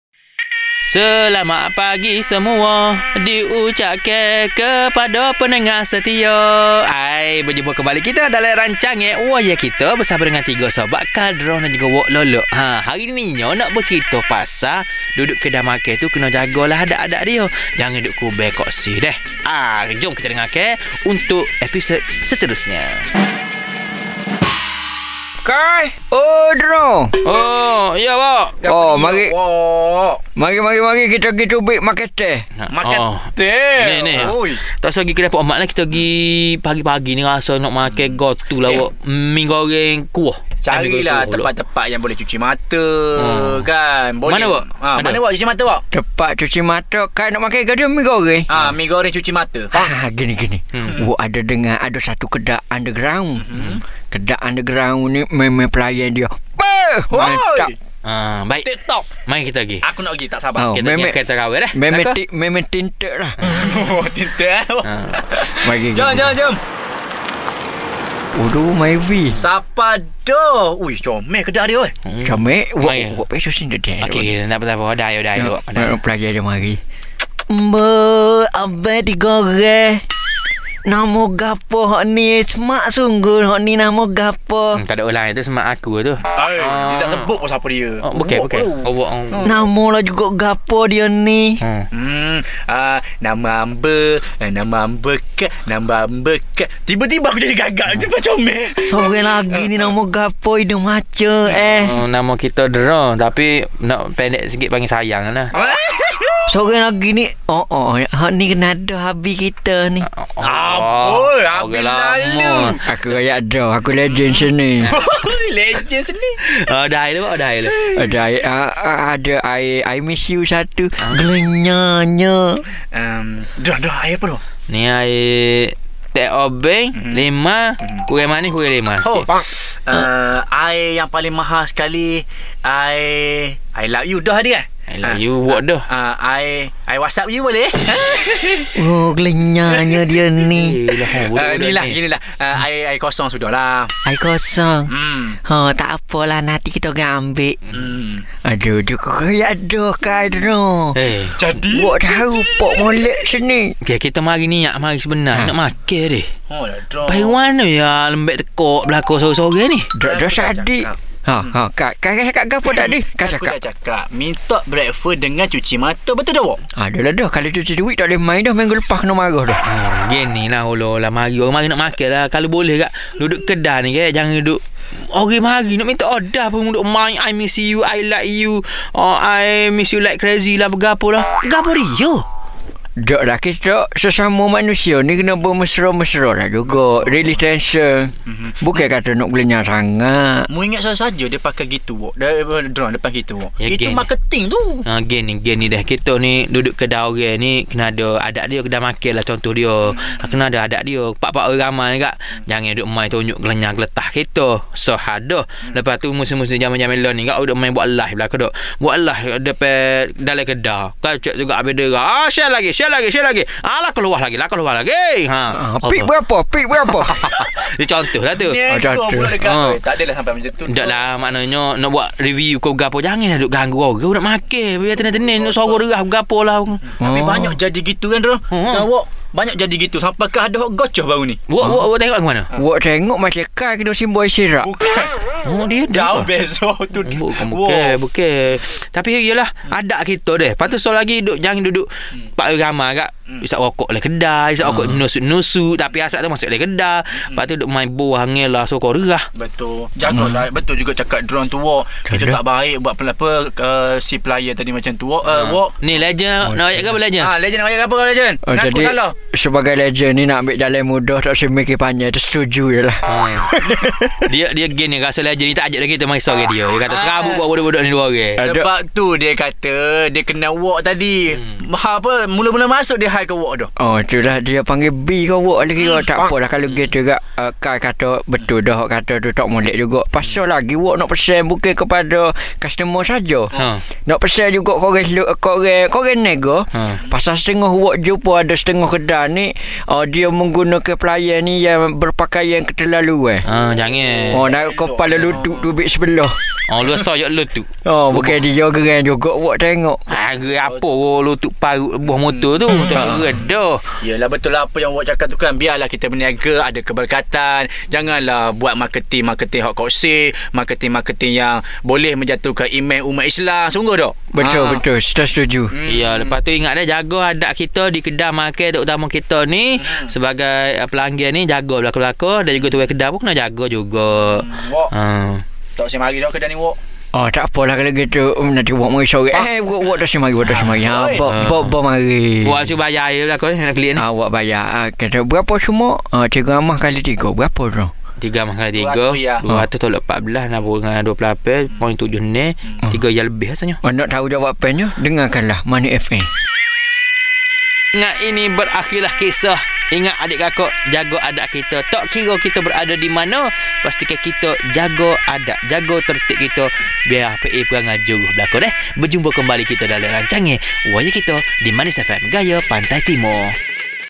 Episod 65 Woye Kito : Adab di Kedai Makan
Pakat dengor belako gapo hok Wady, Ezaq & Kai nok pesan dalam Woye​ Kito mengenai Adab di Kedai Makan.